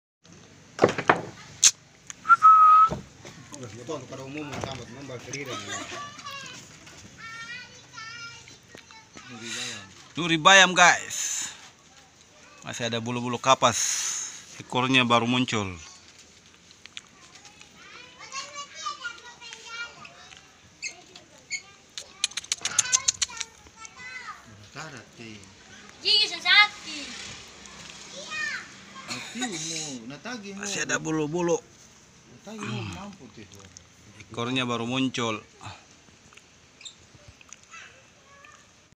MASNAIT TV..ANAK NURI BAYAM YANG LUCU
Di perkirakan anak nuri bayam ini usianya bru 2 bulan sehingga belum banyak bulu.